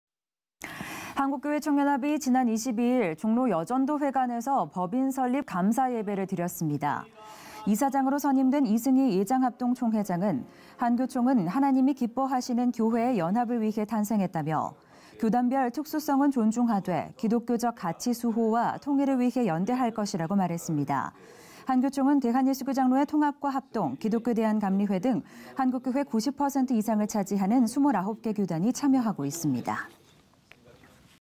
한교총 법인설립 감사예배 드려
한국교회총연합이 지난 22일 종로 여전도회관에서 법인설립 감사예배를 드렸습니다.